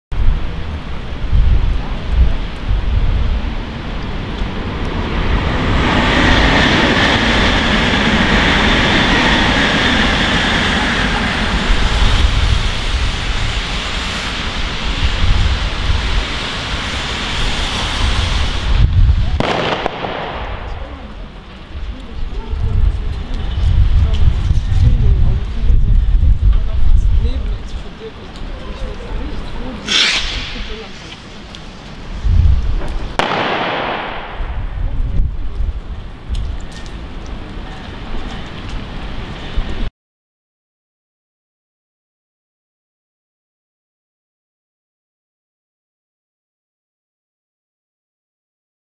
[Polizei & Rakete] Galerie Troisdorf
Den ganzen Abend schon spielt sich das gleich Spiel wie gestern ab. Und ich glaube es noch nicht mal der letzte Tag.

Und wie man an der Tiefgarage der Galerie sieht, steht dort ein Streifenwagen der Polizei und dahin wird dann eine Rakete abgeschossen.

Was soll man noch dazu sagen? Härtere Strafen ?
Was sagt Ihr dazu ?

Wünsche Euch viel Spass mit dem Video.

Herzlichst Euer Markus

Wer meine Kampagnen & Projekte unterstützen möchte

Galerie: ▶️ xxx

▶️ Mein Equipment: ◀️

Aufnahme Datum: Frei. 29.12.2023
Video erstellt Dat.
Text: MB
Foto:
Sprecher:
Kamera: MB